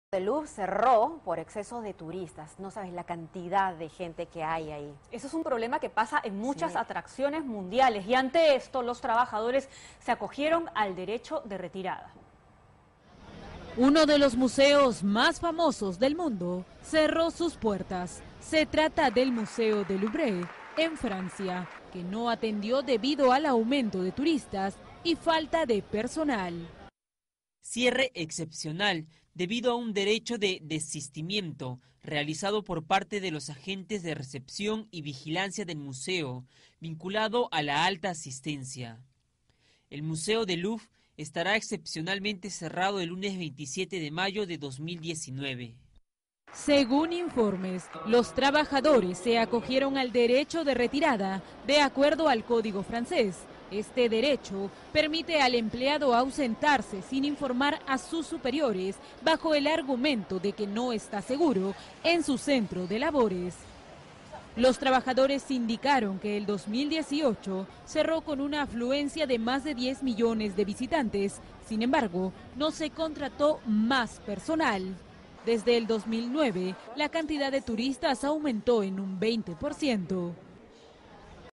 París: Museo de Louvre cierra por exceso de turistas
Demasiada popularidad. El crecimiento y la avalancha de popularidad ha podido con la salud de los trabajadores y las bajas por agotamiento han sido tan numerosas, tras este fin de semana, que la dirección del Museo de Louvre, en Francia, ha comunicado, de manera inesperada, su cierre.

El centro se disculpa ante los que tenían programada la visita y señala que las entradas vendidas serán reembolsadas. Además, avisa de que el flujo que se prevé para los próximos días, tras este lapso, será mucho mayor de lo habitual, por lo que solo garantizan el acceso con la reserva en su web.

En 2018, el Louvre obtuvo un récord histórico de visitas, consiguiendo 10.2 millones de personas. Con esa cifra, el museo creció un 25% respecto al año anterior, es decir, alguien entra cada dos segundos, y cada día más de 20.000 visitantes se asoman a la estancia donde se conserva La Gioconda, de Leonardo da Vinci.

Las críticas ante el cierre señalan el número insuficiente de vigilantes y reclaman al Ministerio de Cultura la contratación de más personal, para atender el flujo multitudinario. Desde su nombramiento en 2014, el presidente del Louvre ha invertido cerca de 60 millones de euros en mejorar los accesos, reducir las filas y acelerar las entradas y salidas al recinto, además de fomentar la venta por Internet.